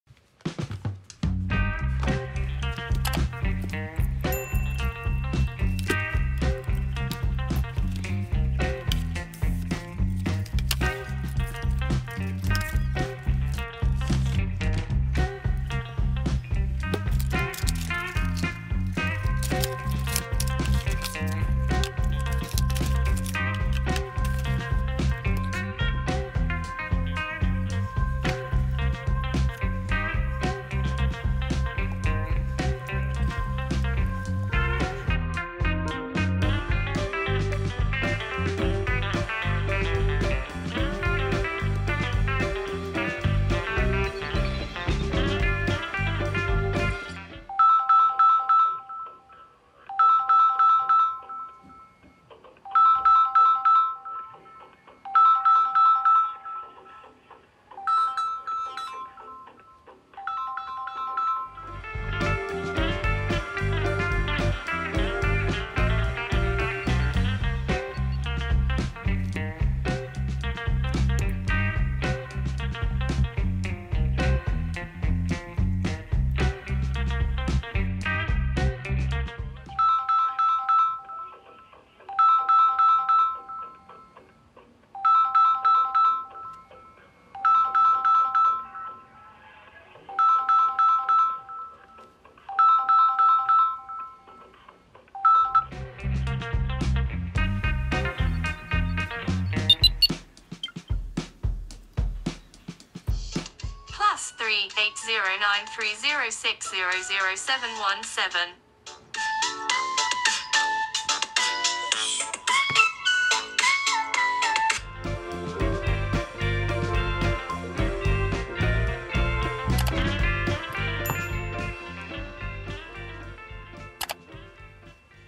Turbo Unboxing Samsung A14 Incoming Call & Outgoing Call + Galaxy Z Flip 3
Fast Unpacking Samsung Galaxy A14 Incoming Call & Outgoing Call + Samsung Galaxy Z Flip 3